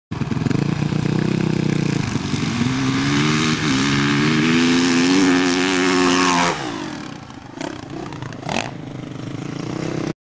M2U04730.MPG
Earthquake Industries Shaun Richardson jumping 190 foot to the back wheel in 3rd gear on a 450.